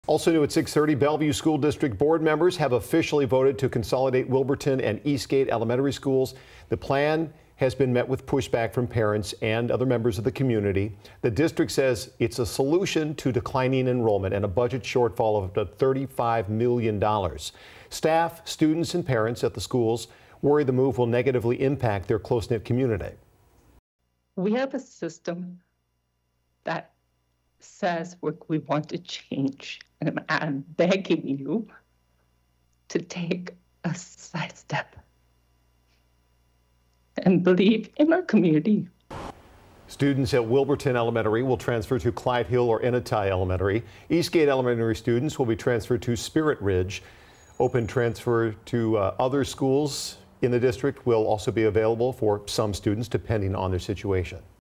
Bellevue School Board votes on school consolidation plan
The Bellevue School Board voted Thursday to consolidate two elementary schools over concerns about declining enrollment.

Three board members voted to approve the plan and two abstained from voting.

Wilburton and Eastgate elementary schools will be consolidated, and students and staff will be sent to other schools in the district.